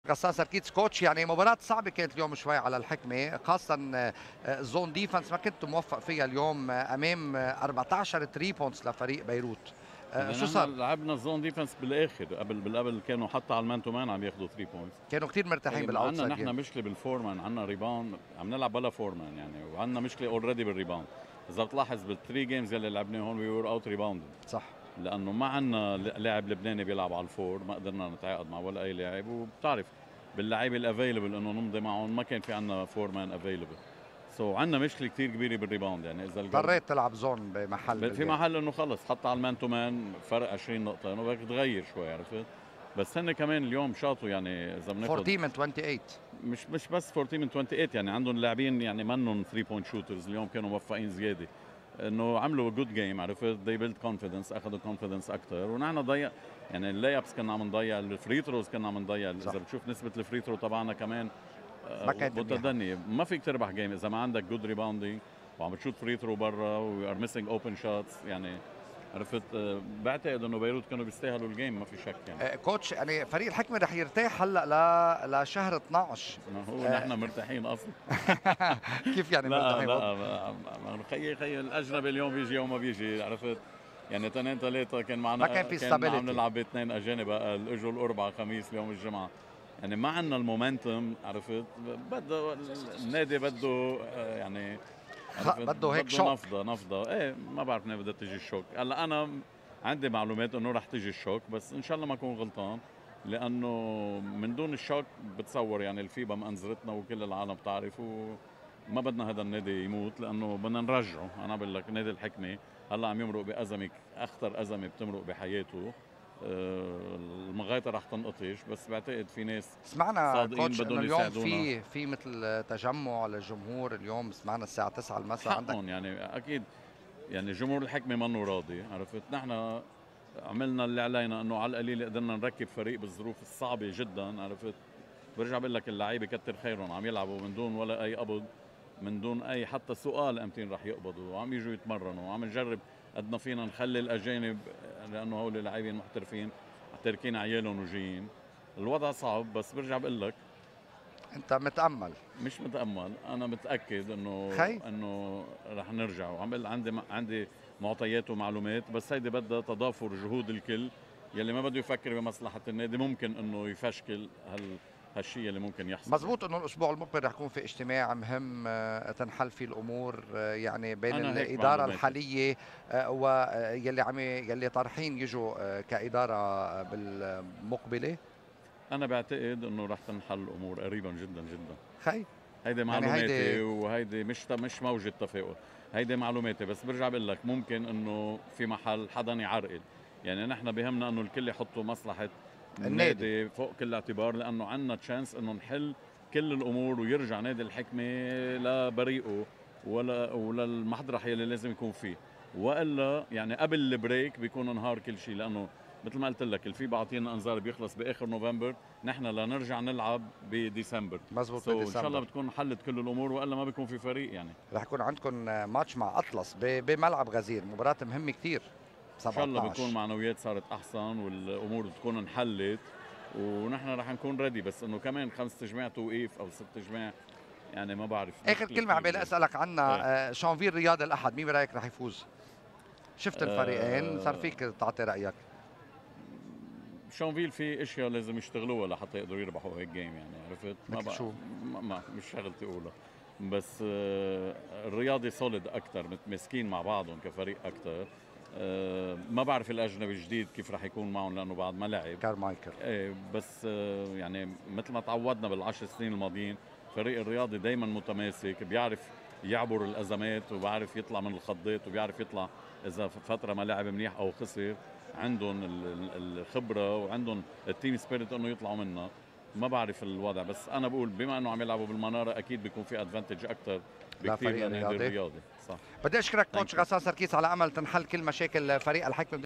Alfa Basketball Championship - Sagesse v Beirut - Ghassan Sarkis Post Game Interview
بيروت يفوز على الحكمة بنتيجة 90-67 في إطار المرحلة الثالثة من ألفا بطولة لبنان لكرة السلة